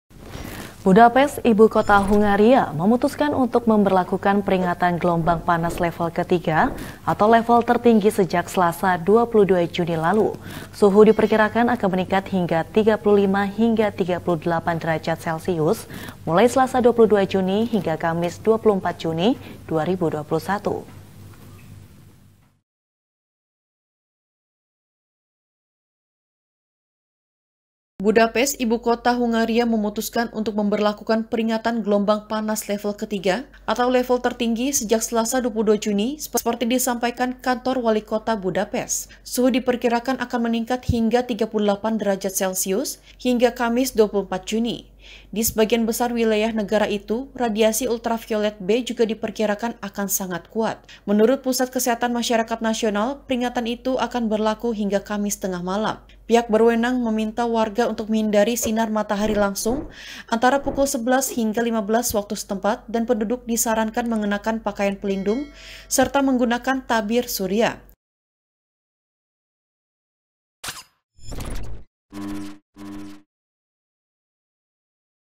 Budapest Dihantam Gelombang Panas, Pemerintah Hongaria Terbitkan Peringatan Level 3
- Budapest ibukota Hungaria, memutuskan untuk memberlakukan peringatan gelombang panas level ketiga, atau level tertinggi sejak Selasa 22 Juni lalu. Suhu diperkirakan akan meningkat hingga 35 hingga 38 derajat selsius, mulai Selasa 22 Juni hingga Kamis 24 Juni 2021.